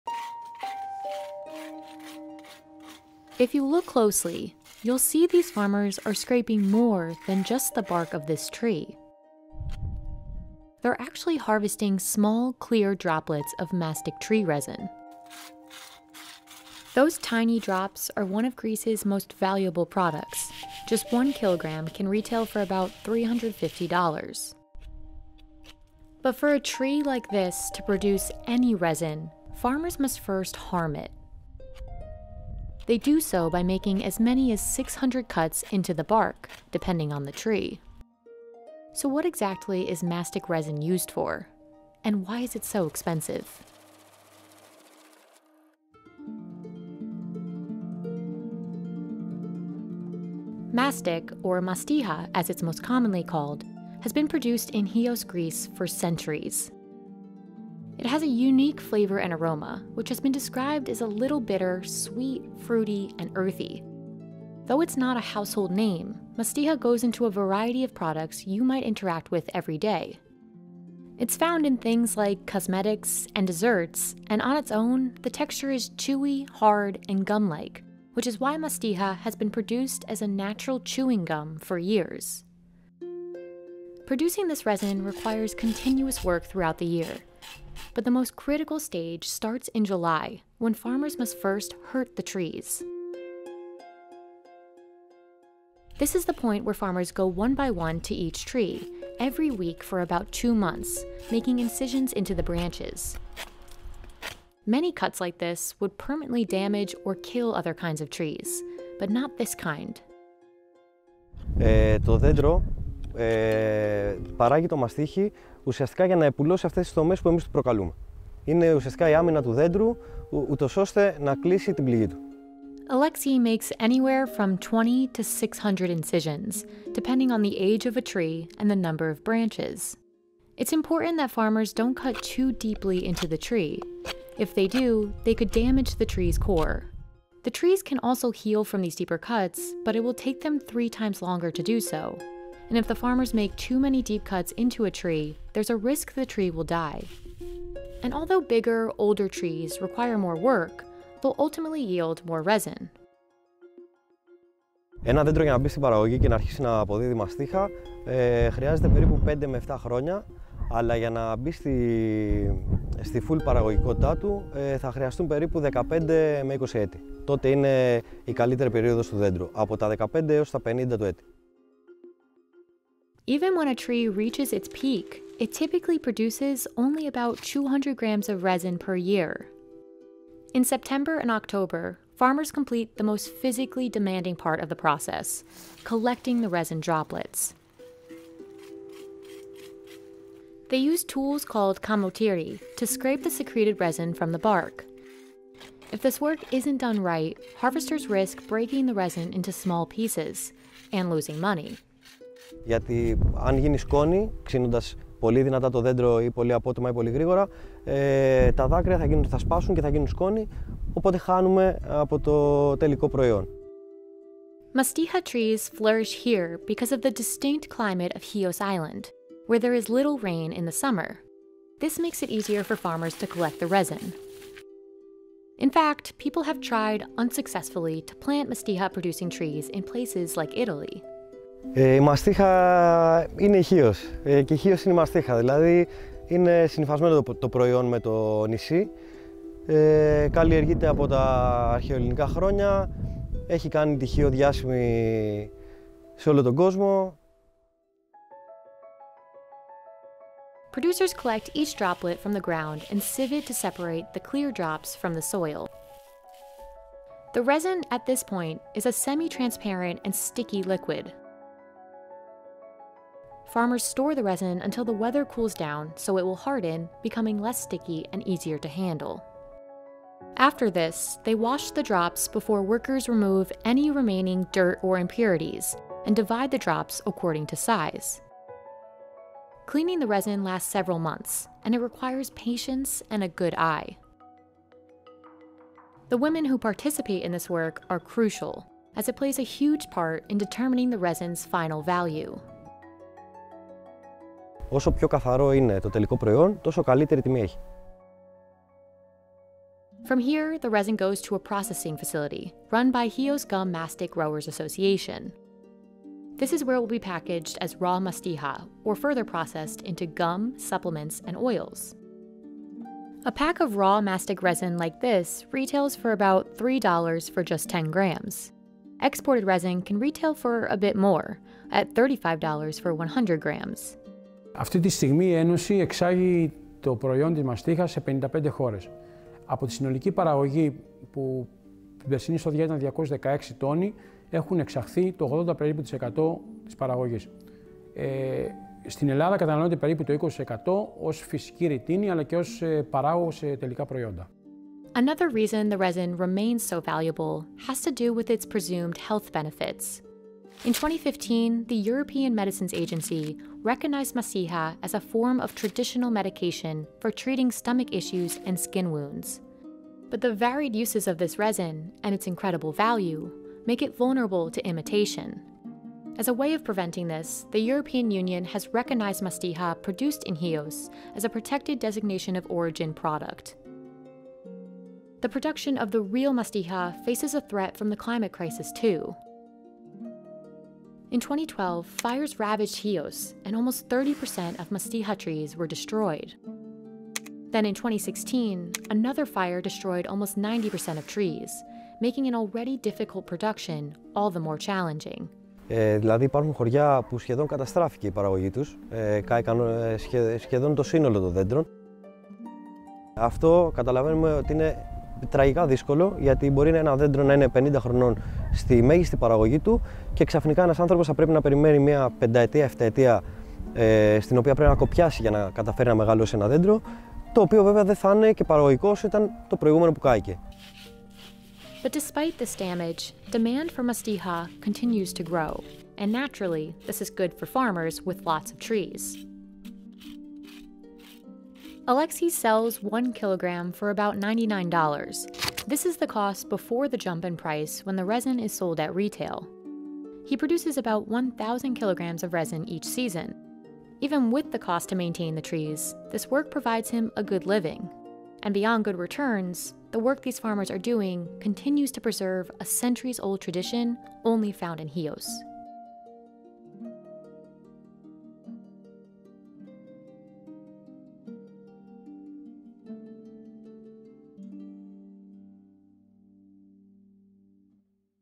Why Mastic Tree Resin Is So Expensive
Greece is famous for the production of mastiha, or mastic, a tree resin collected from mastic trees that flourish in the distinct climate of Chios. Just 1 kilogram can retail for about $350. Today, it's known as "white gold" for its presumed health benefits and various uses in gum, cosmetics, and food. But harvesting this resin isn't simple. It requires farmers to first hurt the trees with as many as 600 cuts into each one's bark. It's only after each droplet has been collected by hand that the resin can be turned into products used around the world. So, what exactly is mastic resin used for? And why is it so expensive?